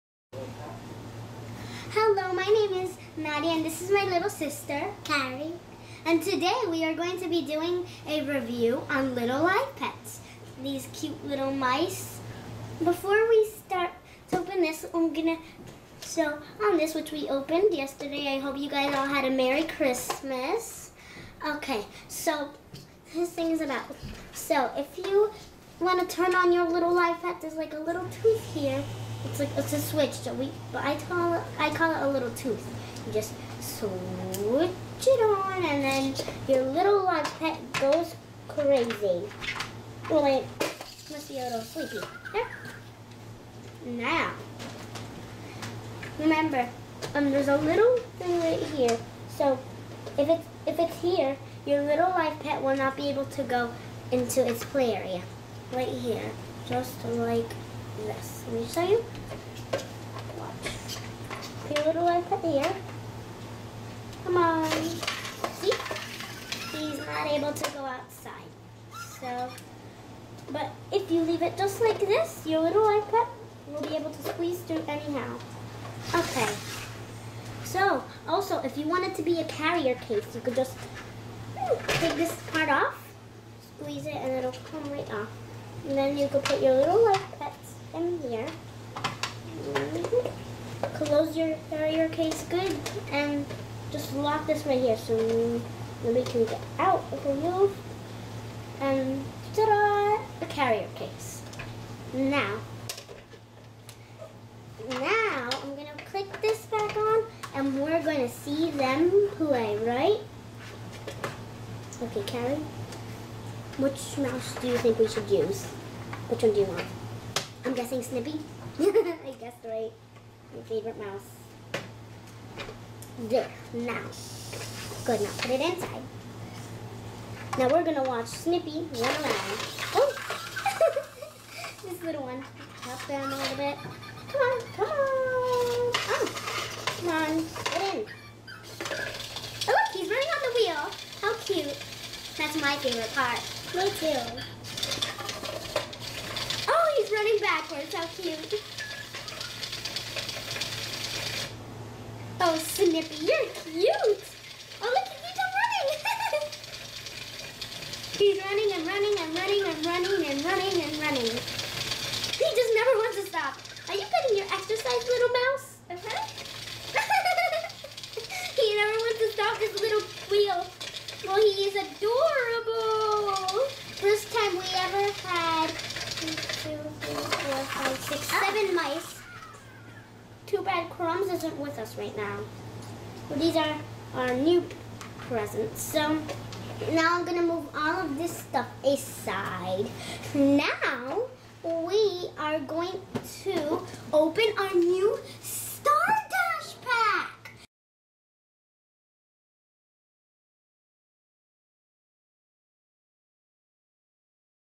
Nadia and Carrie's Little Live Pet Review, Part 1 of 3
In Part 1, watch Nadia and Carrie review and play with the small Little Live Pets set.  Then in Part 2, watch Nadia and Carrie open and set up the big box playset that includes the Little Live Pet mouse - Stardash.  Finally in Part 3 they play with ALL of their pets in the house cages!!!